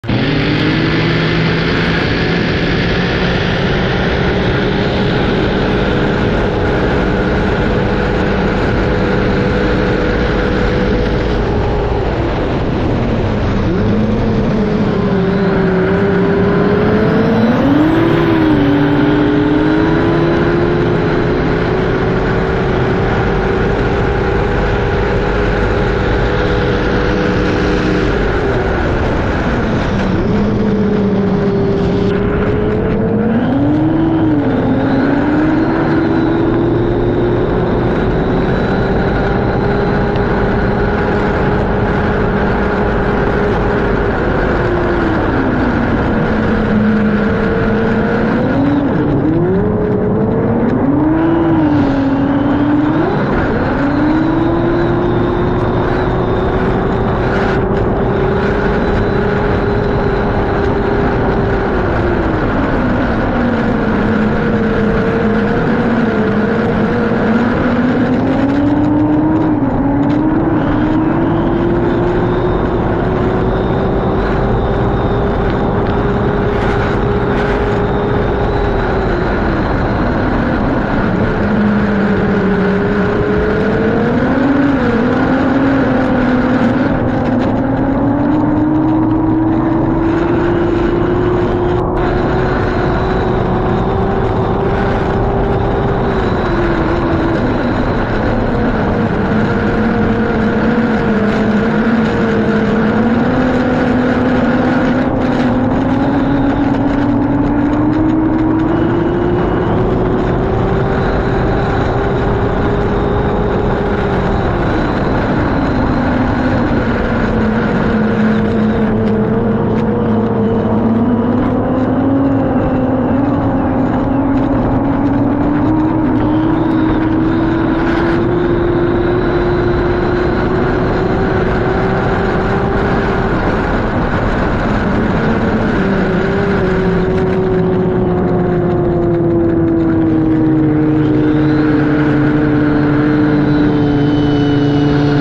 Australian Horsepower vs American Horsepower on Water!
Dodge R5P7 vs Ford Barra Turbo on water, same boat.